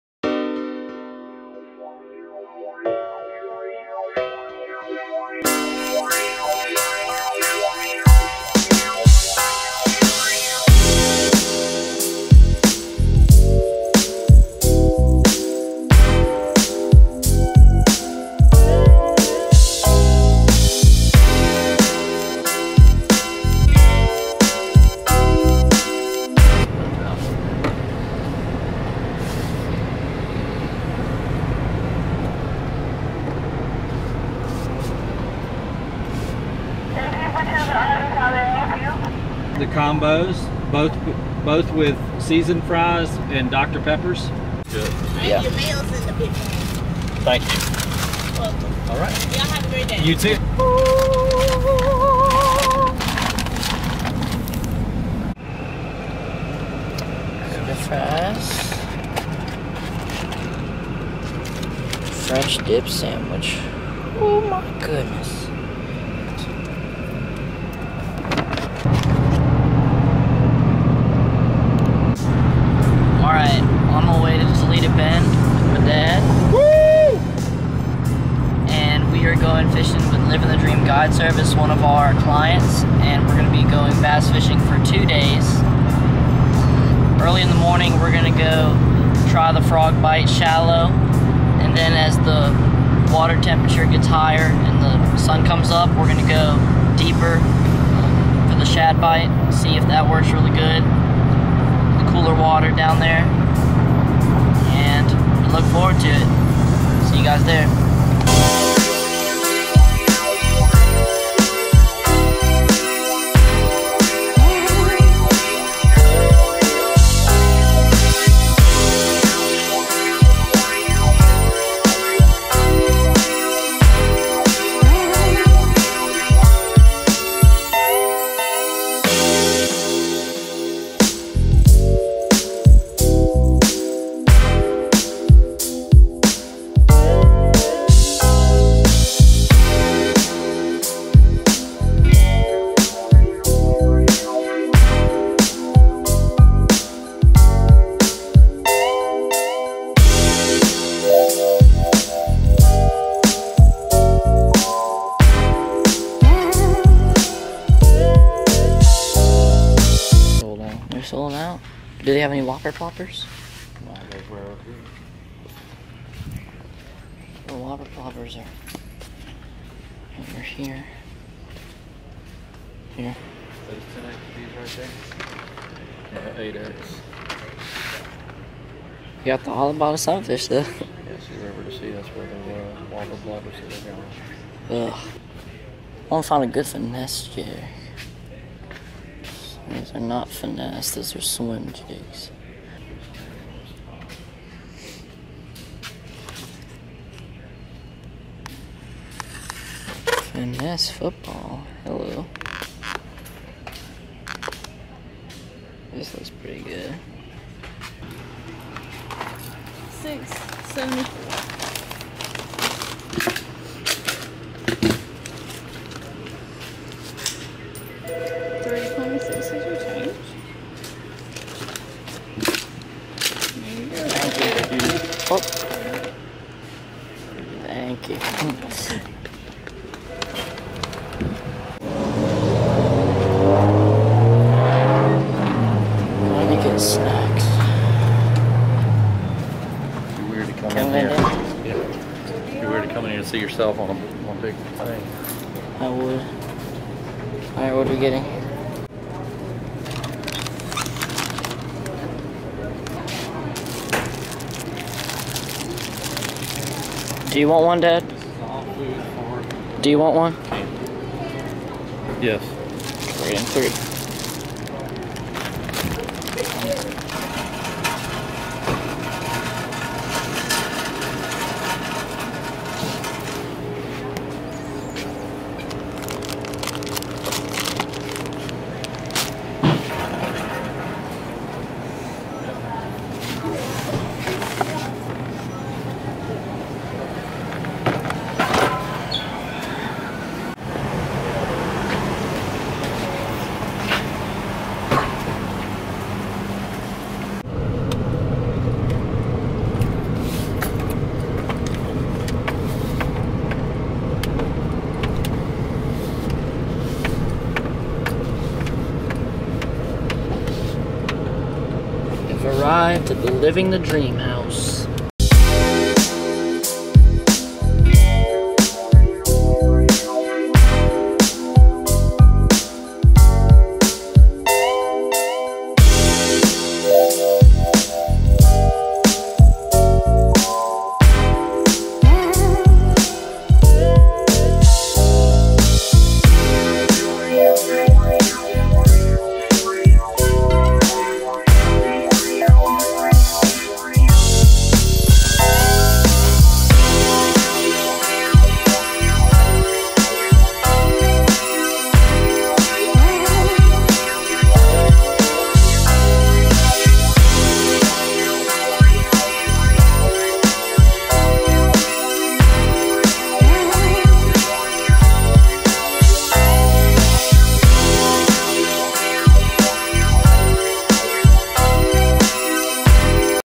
Vlog #2 (Journey to Toledo Bend)
In this video my dad and I travel to Toledo Bend Reservoir thanks to Living the Dream Guide Service.